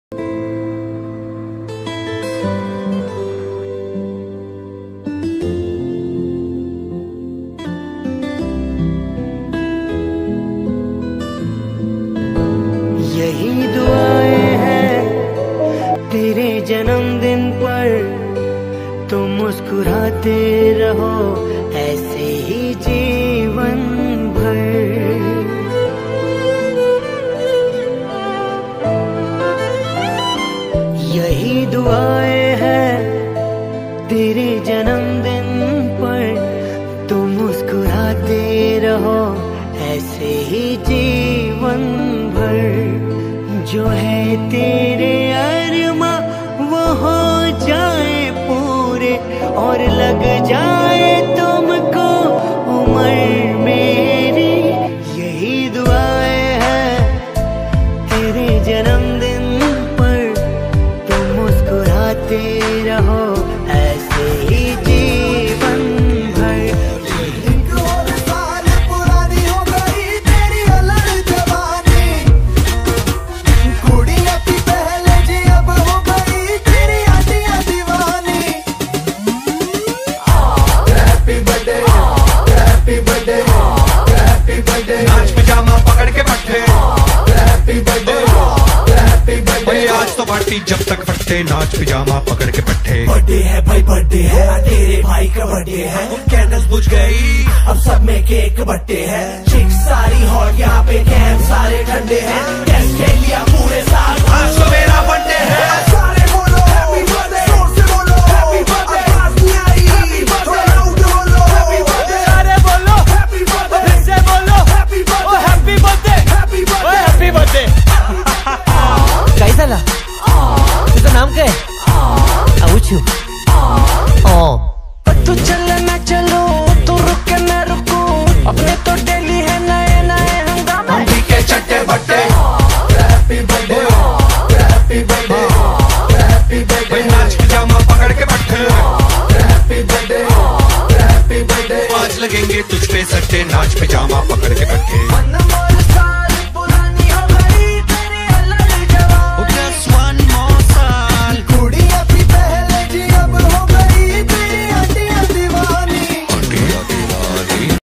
Happy Birthday Mack Sir - Mack Dance Cultural Institute
happy birthday sir this is the small documentary for memory